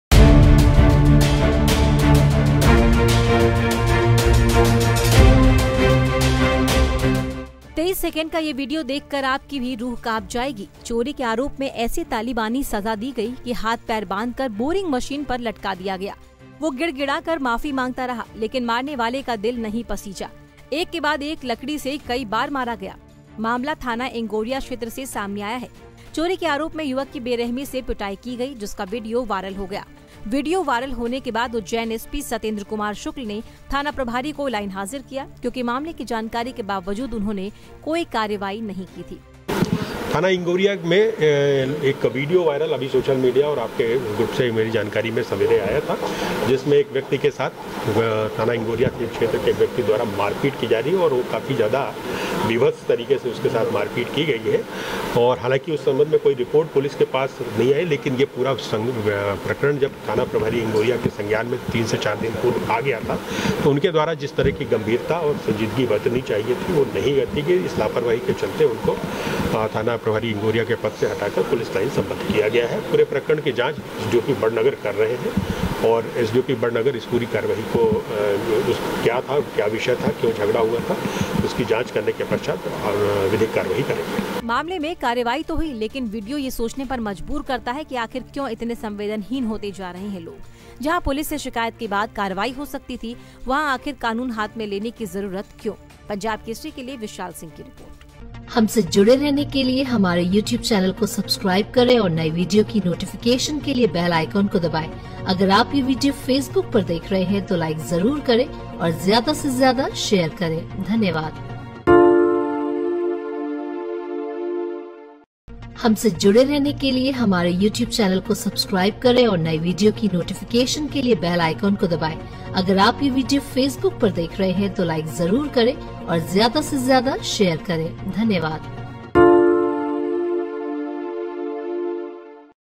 हाथ पैर बांध बोरिंग मशीन पर लटकाकर पीटा...चोरी के आरोप में दी सजा...वीडियो वायरल होने पर कार्रवाई..
Also Watch ►